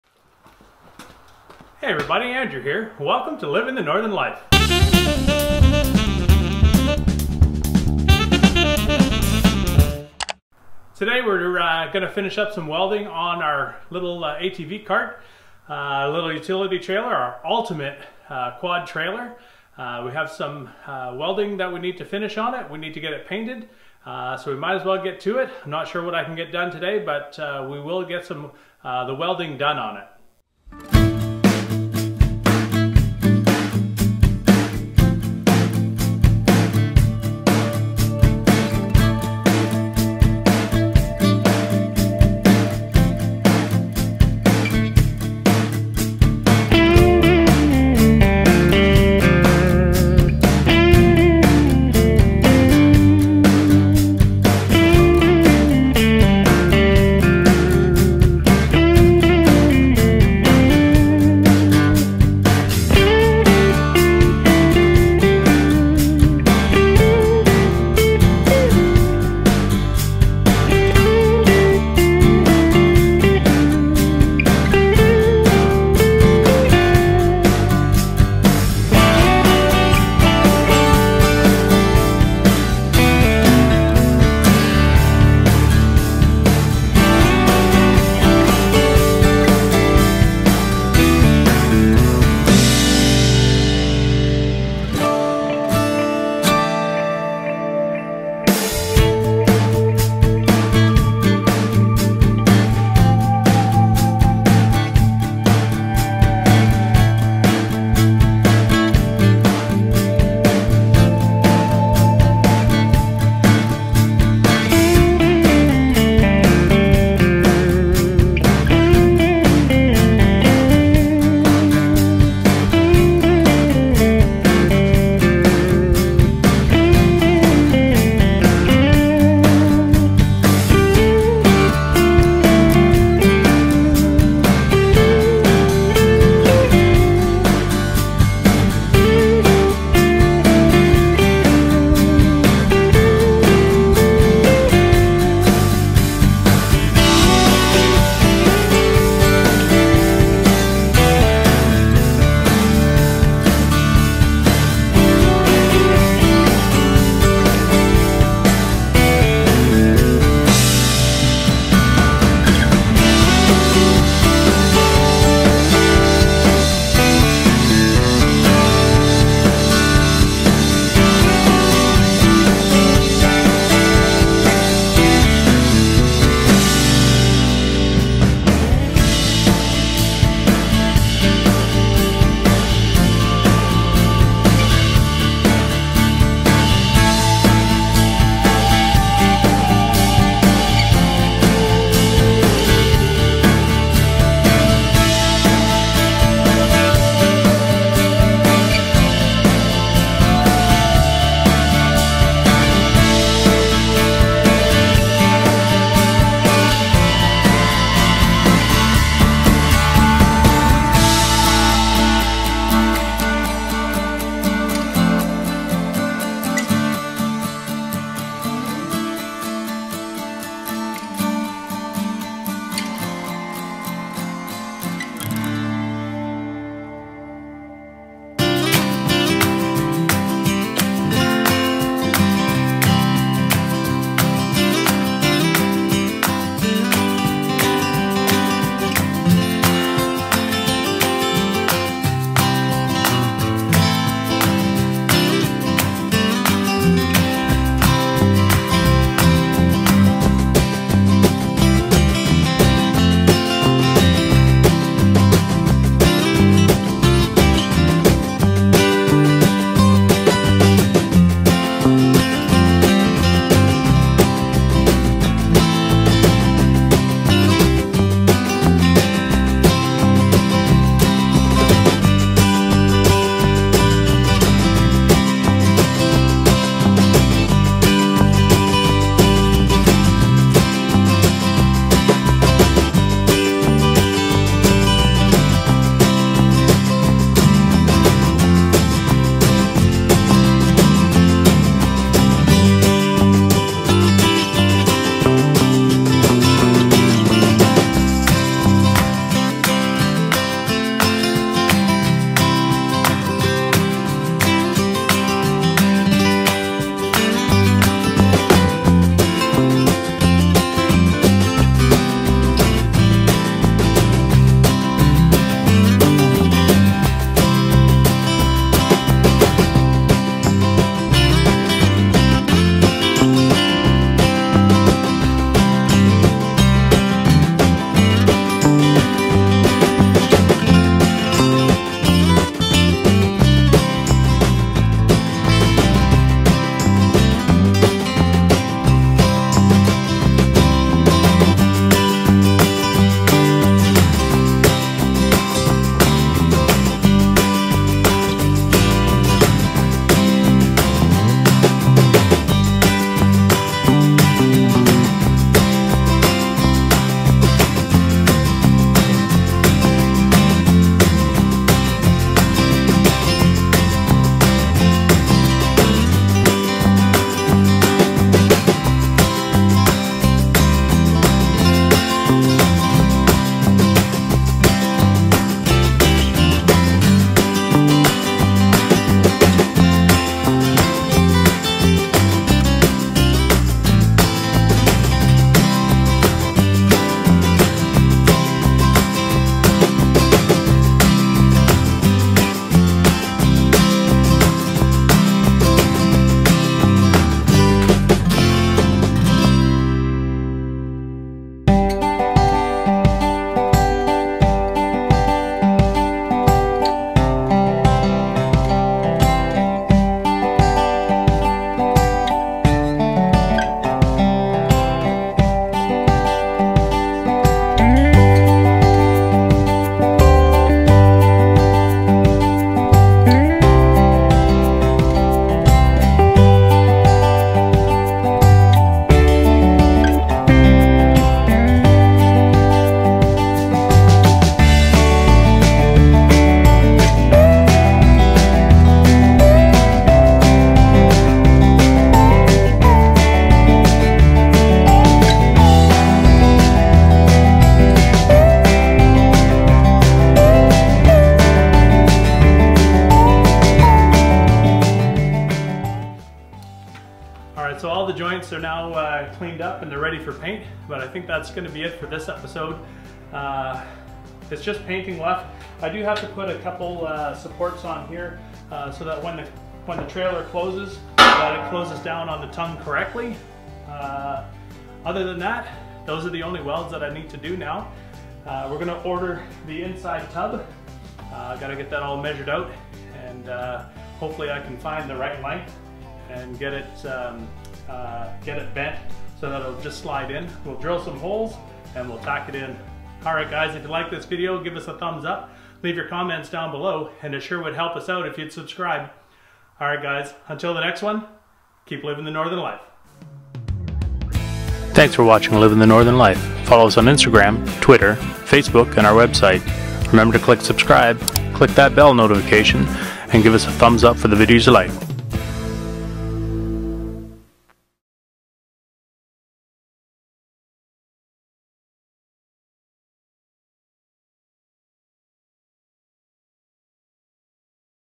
The Ultimate Quad Trailer - Part 6 (theultimatequadtrailer)
The welding and grinding is almost finished and the theultimatequadtrailer will soon be ready to paint.
We have to order the metal for the tub, the doors and the guides for the dump box so that is latched to the tongue and won't fall off.
It's very cold outside now and it has delayed the project but we are working on a solution to get it finished.

9 inch Chainsaw Tooth Brush Blade

Books
Learn to Timber Frame

Tools
Swanson Tool SO107 12-Inch Speed Square

0:00 Intro
0:12 Plans
0;31 The Welding Begins
4:43 Grinding the welds smooth
7:36 Closing remarks
8:22 Outro